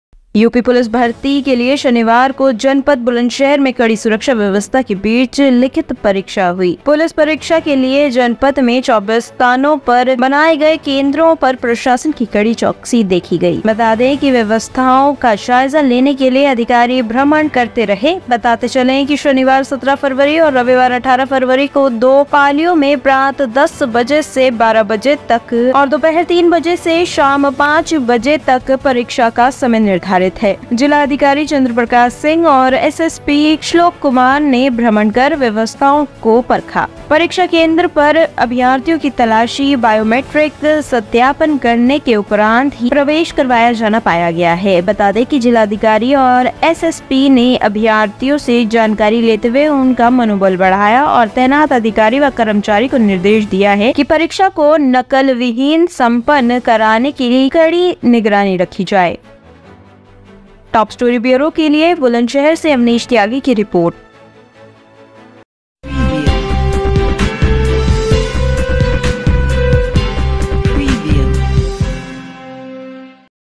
बुलंदशहर में 24 स्थानों पर हुई पुलिस भर्ती परीक्षा अधिकारियों ने परीक्षा केंद्रों का लिया जायजा
बुलंदशहर में 24 स्थानों  पर हुई पुलिस भर्ती परीक्षा अधिकारियों ने परीक्षा केंद्रों का भ्रमण कर लिया जायजा

 यूपी पुलिस भर्ती के लिए शनिवार को जनपद बुलंदशहर में कड़ी सुरक्षा व्यवस्था के बीच लिखित परीक्षा हुई । पुलिस परीक्षा के लिए जनपद  में  24 स्थानों पर बनाए गए  केन्द्रों पर प्रशासन की कड़ी चौकसी रही ।